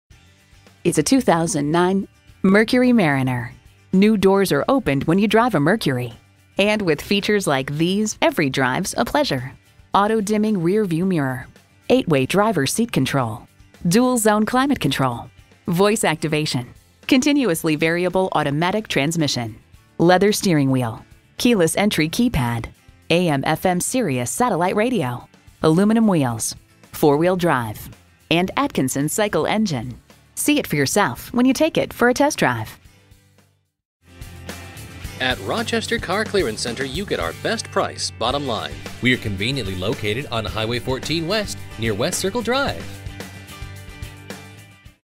2009 Mercury Mariner Rochester Winona, MN FA242038 - SOLD
Year: 2009
Make: Mercury
Model: Mariner
Trim: Hybrid
Engine: 2.5L I4 Atkinson-Cycle 16V iVCT
Transmission: eCVT
Color: Black
Interior: Graystone
Mileage: 95636
Stock #: FA242038
VIN: 4M2CU39359KJ18167
Recent Arrival! 2009 Black Mercury Mariner Hybrid This Mercury Mariner has many features and is well equipped including.brbrCARFAX One-Owner. Clean CARFAX. 29/27 City/Highway MPGbrbrThank you for taking the time to look at this handsome-looking 2009 Mercury Mariner. We look forward to earning your business!

Tune-Ups
Transmission Service
Brake Service
Performance Parts
Factory Wheels

Address:
2980 HWY 14